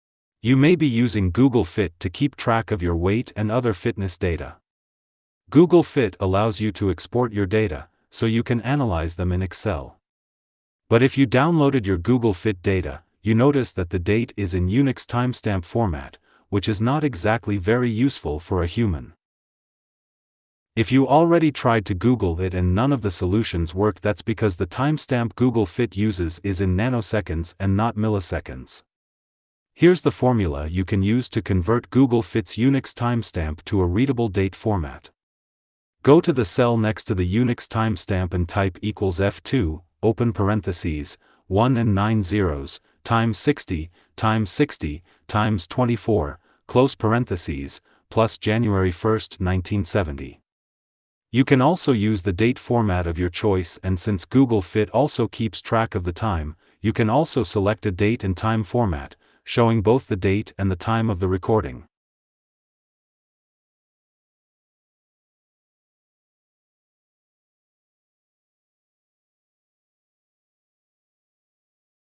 Change Google Fit's TimeStamp to a readable Date format in Excel
Change Google Fit's TimeStamp to a readable Date format|| Change the time stamp of your Google fit data to a readable and understandable date and time format

You may be using Google fit to keep track of your weight and other fitness data. Google fit allows you to export your data, so you can analyze them in Excel.

But if you downloaded your Google fit data, you noticed that the date is in Unix timestamp format, which is not exactly very useful for a human.
If you already tried to Google it and none of the solutions worked that’s because the timestamp Google fit uses is in nanoseconds and not milliseconds.

Here’s the formula you can use to convert Google Fit’s Unix timestamp to a readable date format. Go to the cell next to the Unix timestamp and type equals F2, open parentheses, 1 and 9 zeros, times 60, times 60, times 24, close parentheses, plus 1/1/1970. =F21/(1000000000*60*60*24)+"1/1/1970"

You can also use the date format of your choice and since Google fit also keeps track of the time, you can also select a date and time format, showing both the date and the time of the recording.